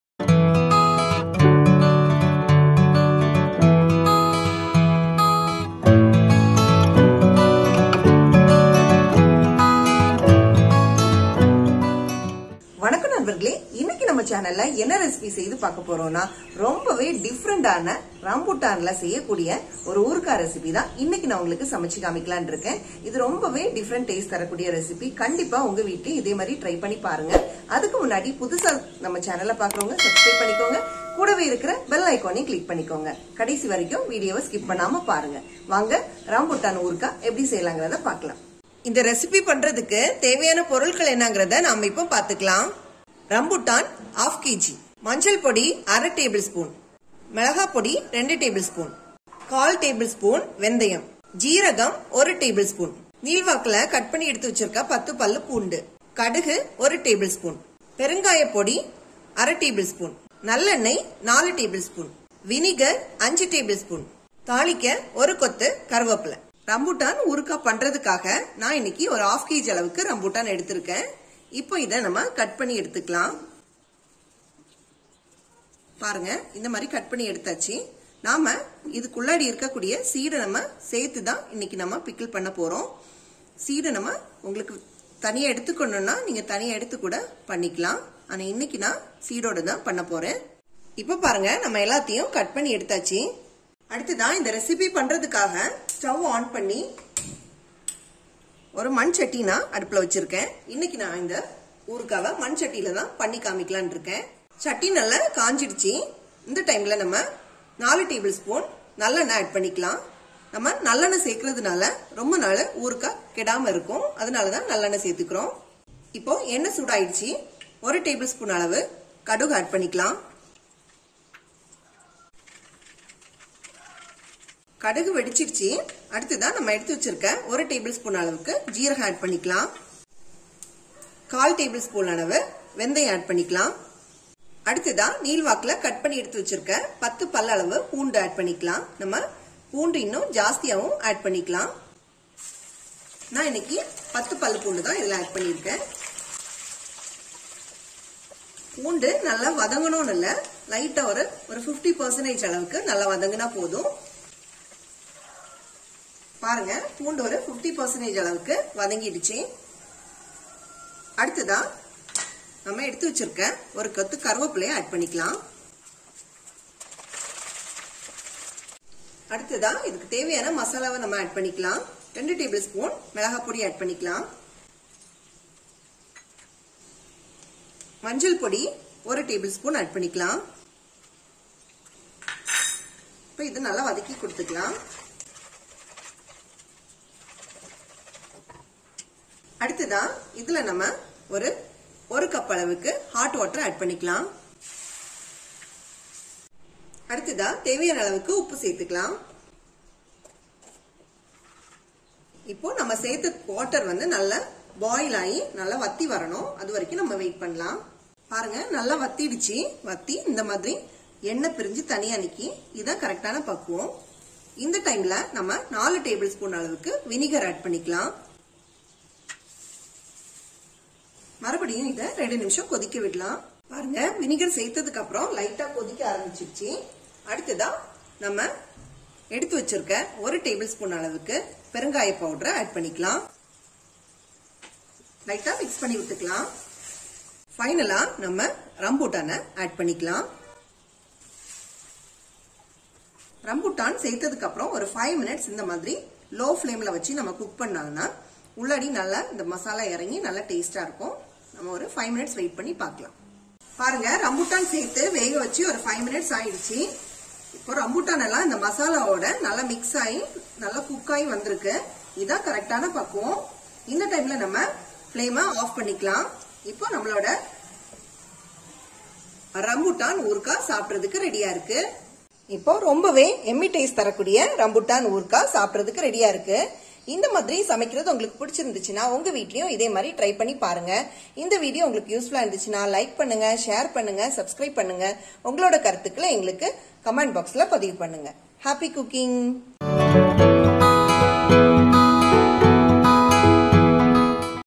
Rambutan Pickle in Tamil | Fruit Pickle | Rambutan Achar | Pickle Recipe in Tamil
Welcome to Village Kumari Paradise!
In this video I am going to show you how to prepare Rambutan Pickle in Tamil. Rambutan is one of the Fruits that produce Seasonally. This Shelled fruit is consumed just like that or also made sweet. It feels rich in taste while eating anytime and anyway. Now Comes a unique choice of eating Rambutan, which is made processed pickled
 Friends try this Recipe at you home & pour your feedback in comment box
Happy Cooking
 Ingredients: -
1. Rambutan – ¾ Kg
2. Turmeric powder
3. Chilli Powder
4. Fenugreek
5. Asafoetida
6. Cumin Seed
7. Mustard
8. Vinegar
9. Gingelly Oil
10. Garlic Cloves
11. Curry Leaves